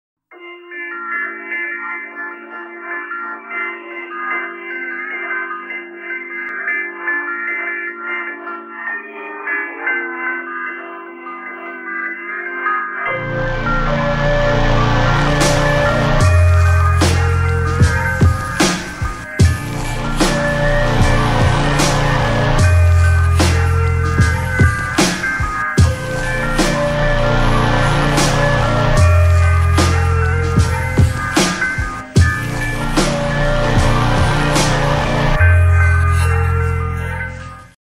Whippet walkies - Timelapse
Standard walk with Mojo whippet in our village and surrounding fields